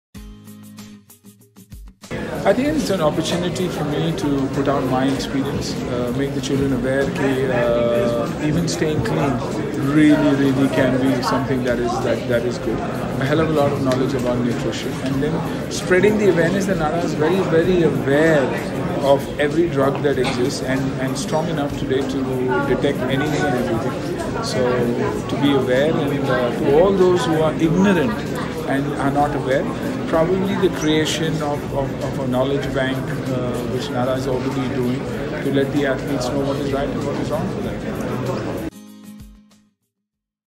Suniel Shetty speaks on his association with NADA
NADA’s Brand Ambassador, Suniel Shetty talks about why it is important to spread awareness about the effects of doping.